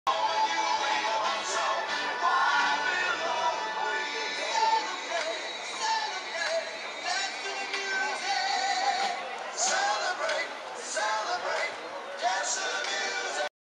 1940 ford
1940 ford at the car show in flowery branch